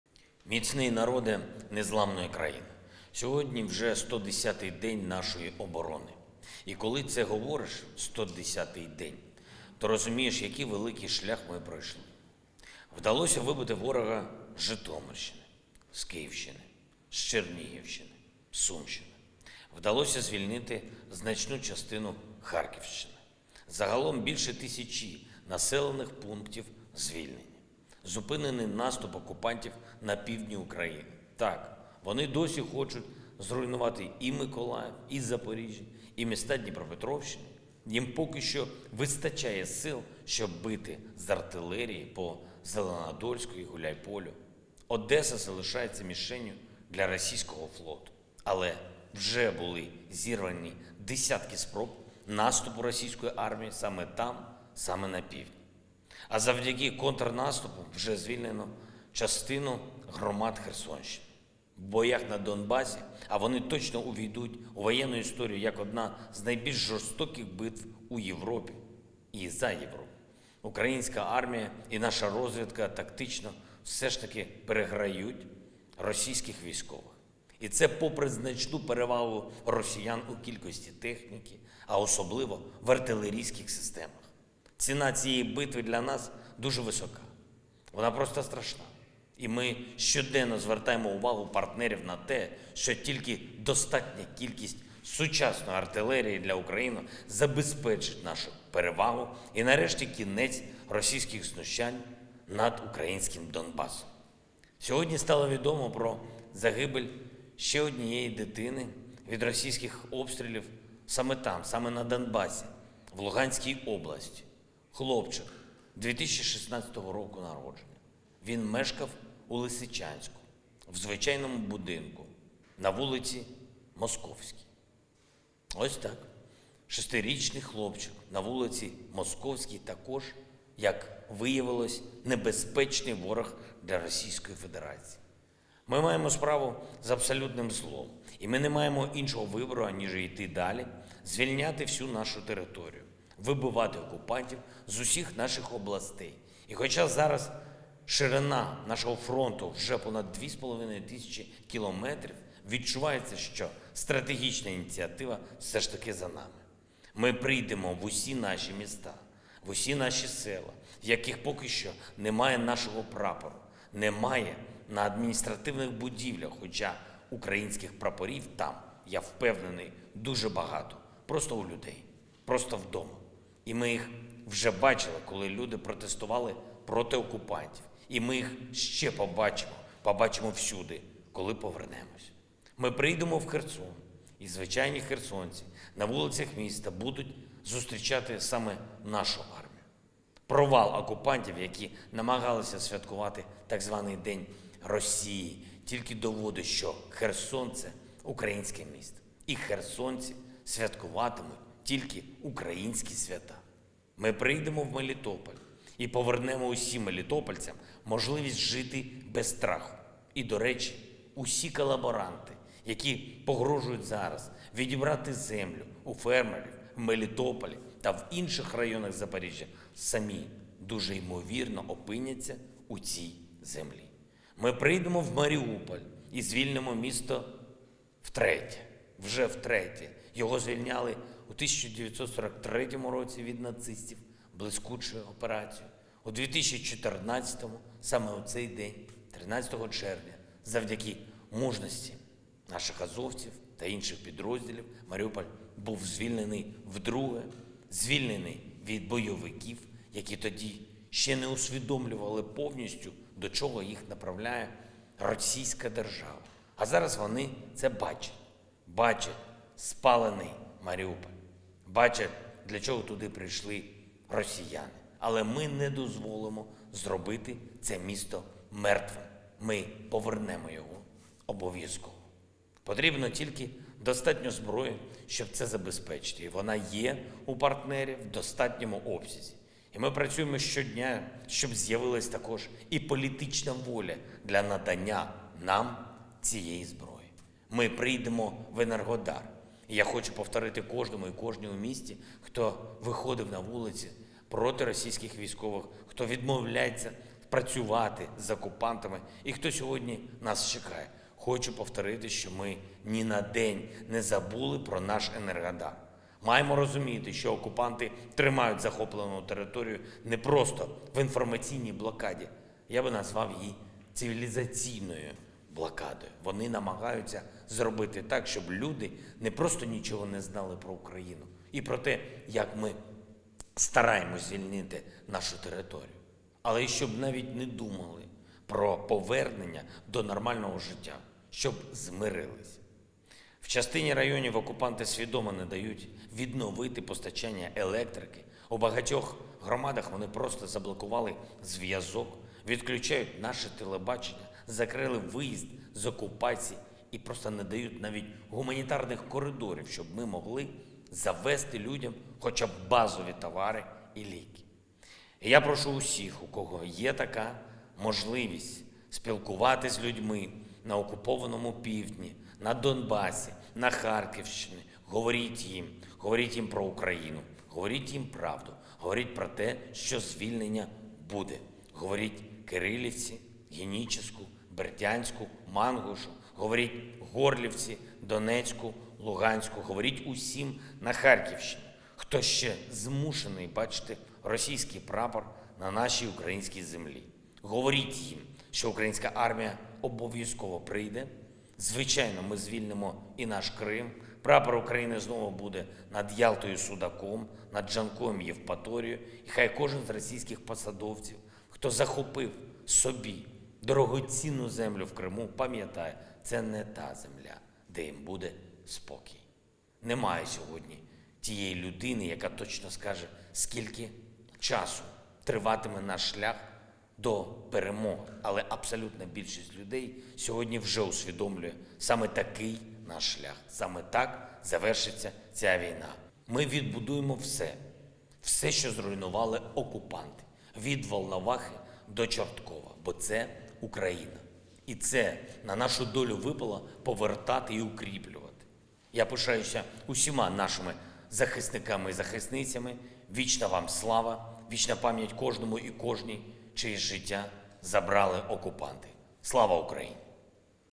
Володимир Зеленський звернувся до українців наприкінці 110-го дня війни
«Ми маємо справу з абсолютним злом. І ми не маємо іншого вибору, ніж іти далі. Звільняти всю нашу територію. Вибивати окупантів з усіх наших областей. І хоча зараз ширина фронту – уже понад дві з половиною тисячі кілометрів, відчувається, що стратегічна ініціатива – все ж за нами.
Ми прийдемо в усі наші міста – Херсон, Мелітополь, Маріуполь, Енергодар, ми прийдемо в усі наші села, у яких поки що немає нашого прапора. Немає на адміністративних будівлях, хоча українських прапорів там, я впевнений, дуже багато – просто у людей, просто вдома. І ми їх уже бачили, коли люди протестували проти окупантів. І ми їх ще побачимо – побачимо всюди, коли повернемось… Ми відбудуємо все, що зруйнували окупанти. Від Волновахи до Чорткова. Бо це – Україна. І це на нашу долю випало повертати й укріплювати».